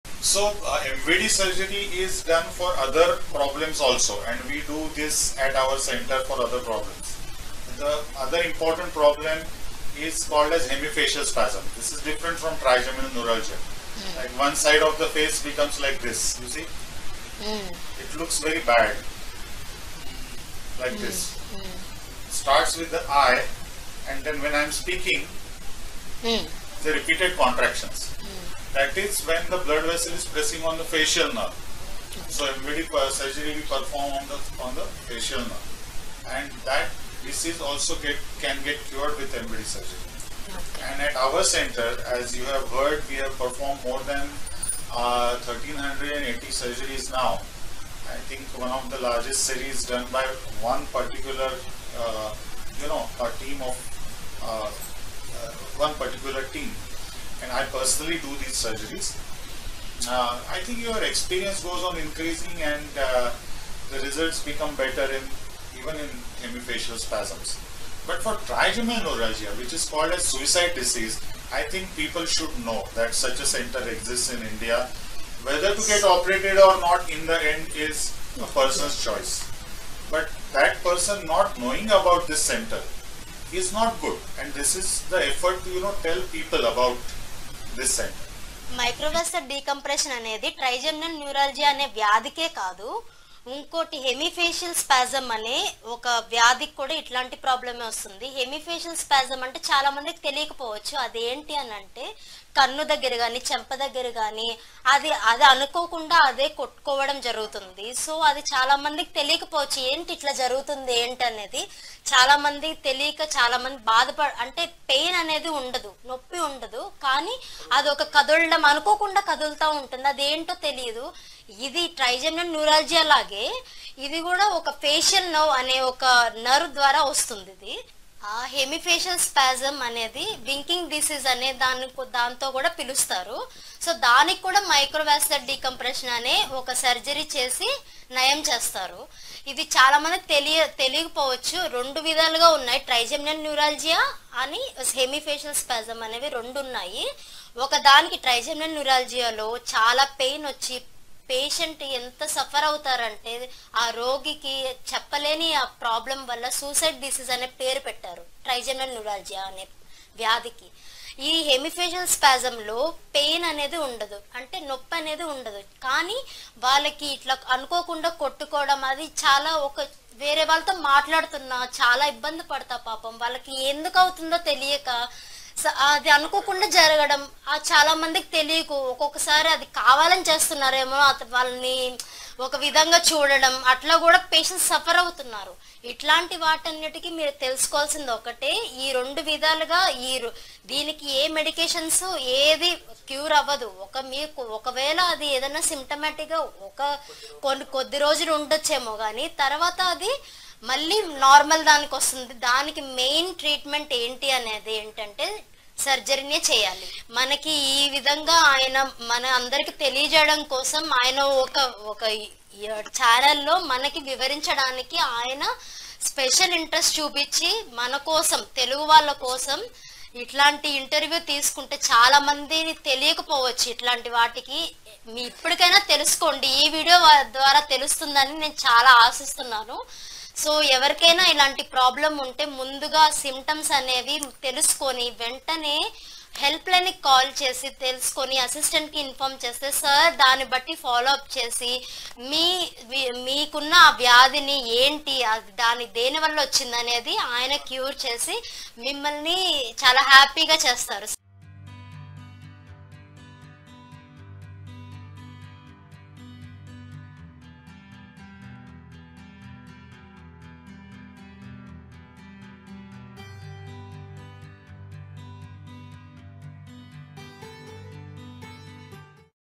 4 of 4. Microvascular Decompression surgery. Dr Jaydev Panchwagh
In part 4 of 4, Microvascular Decompression surgery is discussed by Dr Jaydev Panchwagh.
He explains how MVD surgery may be performed for either Trigeminal Neuralgia or Hemifacial spasms. At the MVD Centre in Pune, MVD surgery is performed by Dr Jaydev Panchwagh who leads the team, since the last nearly 18 years.
Trigeminal Neuralgia is also called Facial pain syndrome. It is a chronic pain disease involving the face. Any simple action like touching, brushing, talking, eating, applying make-up, shaving etc are trigger a wave of pain. Facial pain in Trigeminal neuralgia is typically like electric shock, or electric current or needles stabbing, or knife stabbing. it is so severe and disabling that leading a normal life is impossible. Typically the commonest symptoms of the patients who visit the MVD Centre are: "current shock pain while brushing teeth" or "current shock pain while eating" or "shock like pain while touching face" or "ELECTRIC shock while touching face"...
In Hemifacial spasm, the eye closes without control, or the complaint is of cheek pulling without control. There is no pain like in Trigeminal Neuralgia.
MVD surgery is performed on the 5th nerve or Trigeminal nerve, for Treatment of Trigeminal Neuralgia; and on the 7th nerve or Facial nerve, for treatment of Hemifacial spasms.

Please consult the doctor personally for treatment. Videos cannot replace consultations.